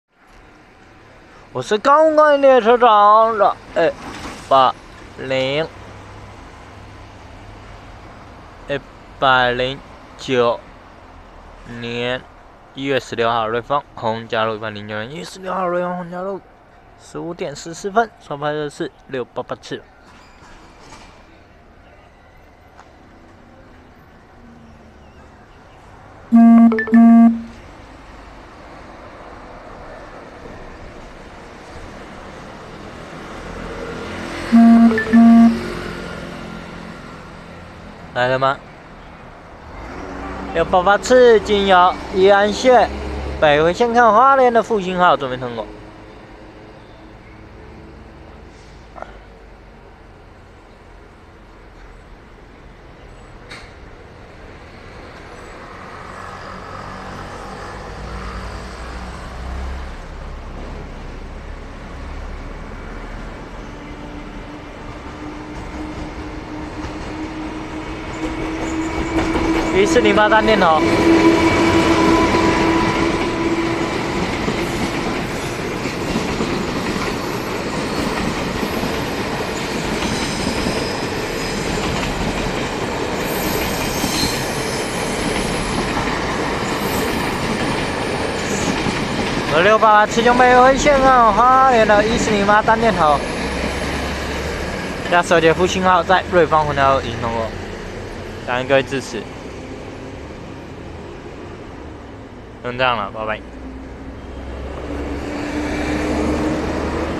688次經由北迴線開往花蓮的E408單電頭+十二節復興號
109/1/16逢甲路15：14分拍攝688次經由北迴線開往花蓮的E408單電頭+十二節復興號